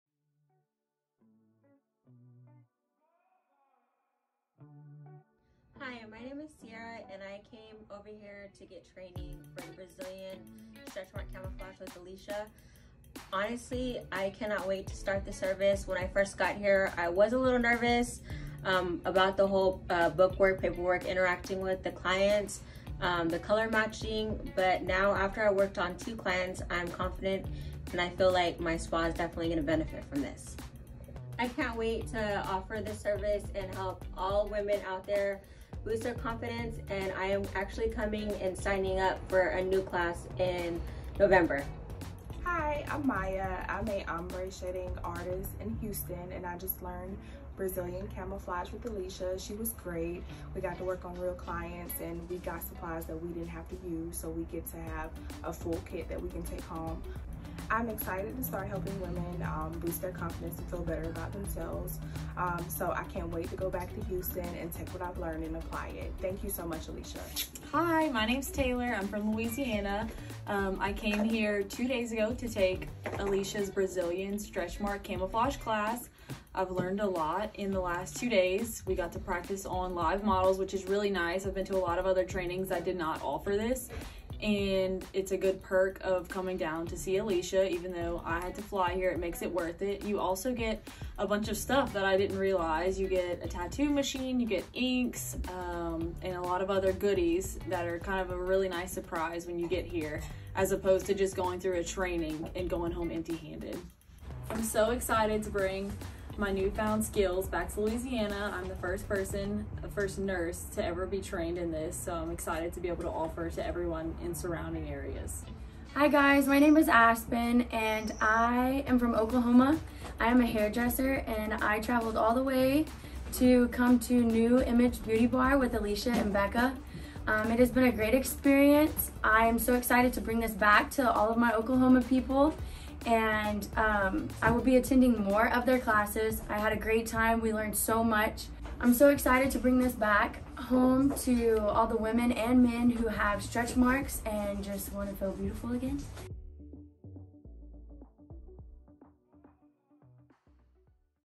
Student Testimonials for Brazilian Stretch Mark Camouflage
We wanted to share some awesome reviews from a few of our students who came in to learn Brazilian Stretch Mark Camouflage.

*All deposits and booking fees are non refundable.* 20% deposit required.

New Image Beauty Bar 🌸
🖥 newimagebeautybar.net